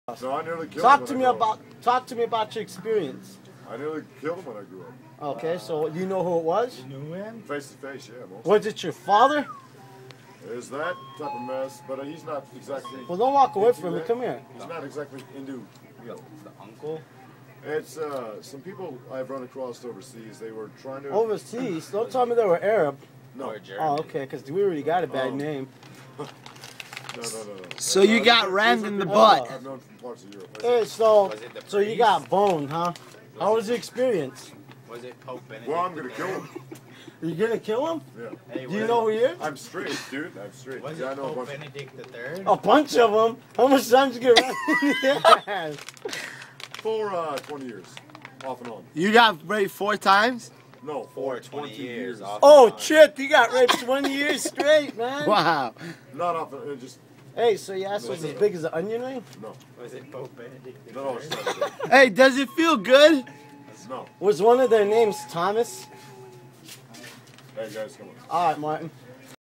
Man got raped
Hes telling his story of him getting raped